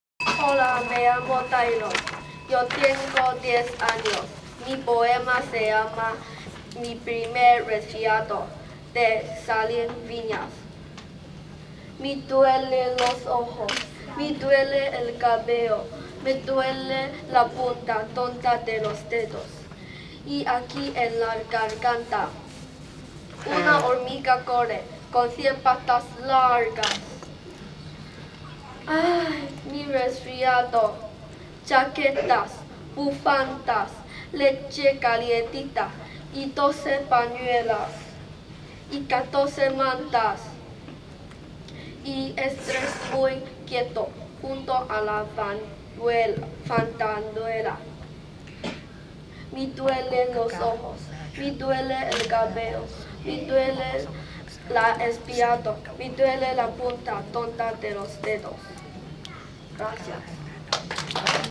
第十四屆超級語言朗誦比賽 04 李承峻 小學四年級 西班牙文詩歌
第十四屆超級語言朗誦比賽(第一場)

比賽地點 : 尖沙咀文化中心AC1